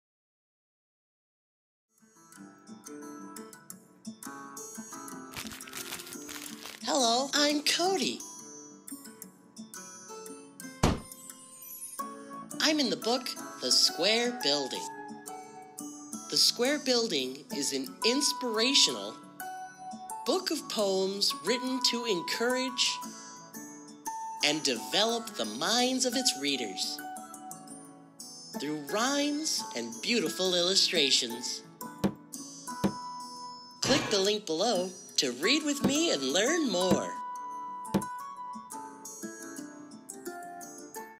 The Square Building - Official Book Trailer
The Square Building written and illustrated by Author Justin Jerome Mitchell and officially published by Xlibris Publishing Company is full of poetry and illustrations that your child will love to read or have read to them while learning in the process.

This Stop Animation book trailer was created by JustinJeromeDesigns.com an online design company
The voice over for the main character Cody in the trailer was done by
James R. Smith

The Square Building Can Be Purchased at the following online retail stores.